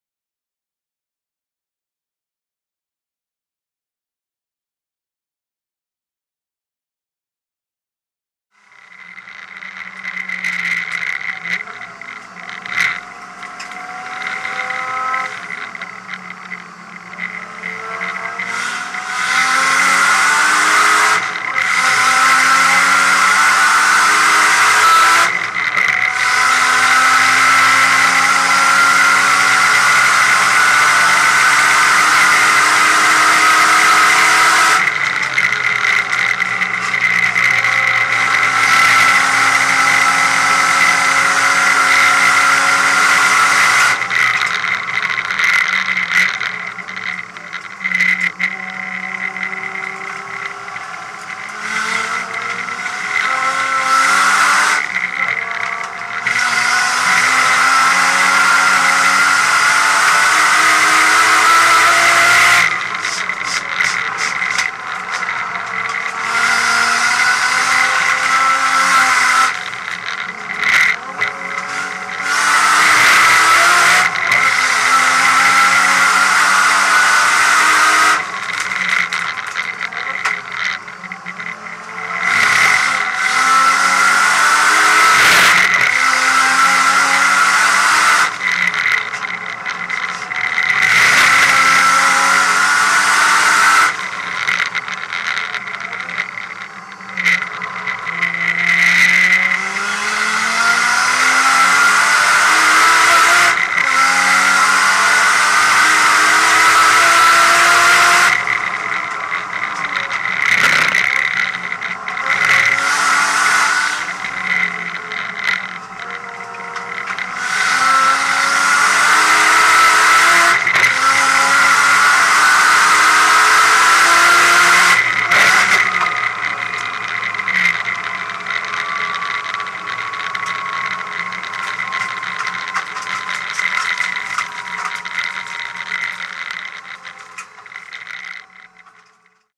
Russman WTAC 2012 blown gearbox
Pretty decent lap until I broke 5th gear. Wonder what time it would have been if I kept accelerating to the line. I think I rolled over the line with a 1:40.2.
Immediately after this session the team swapped the gearbox and got me out for my last session and we recorded a time of 1:38.57